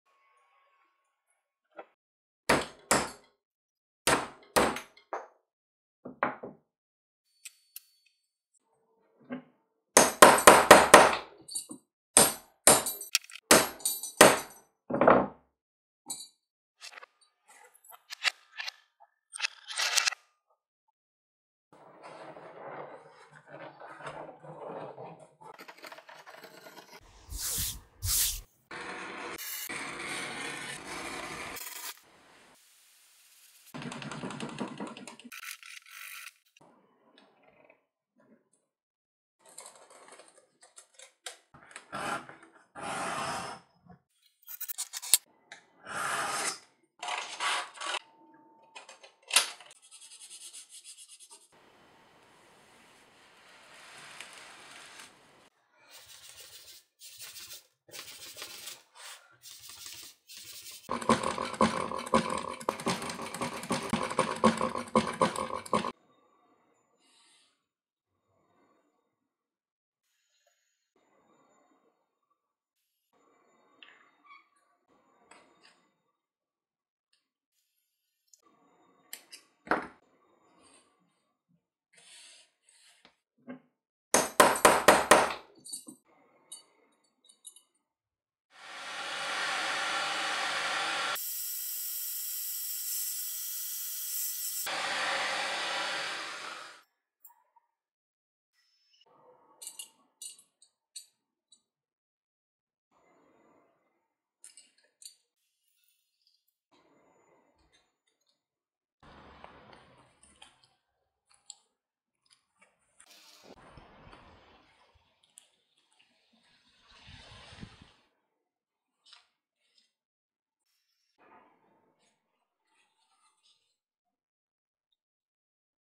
Leather belt | Handmade | Mens belt | ASMR
In this video, I showcase the process of crafting a unique leather belt designed for jeans. The Italian vegetable-tanned leather is hand-dyed, creating an aged effect. The patina and texture of each belt are one-of-a-kind, varying with each creation.

The timeless buckle is crafted from natural brass, available in two options: Classic solid brass or Antique solid brass.

The belt has a width of 1.54 inches and can be custom-made to any length, with the option for self-adjustment using a screw.

This model offers personalization:

The first option includes a logo, monogram, or initials, crafted with a laser engraver. Placement is on the outer end of the belt.

The second option features initials or the customer's name in a classic font, done through embossing. The personalization can be placed on the outer end of the belt or next to the buckle on the inner side.

The third option is without personalization, leaving the belt end empty.

I'll be showcasing my favorite belt, wallet, and bag designs.

Sincerely,
Vitaliy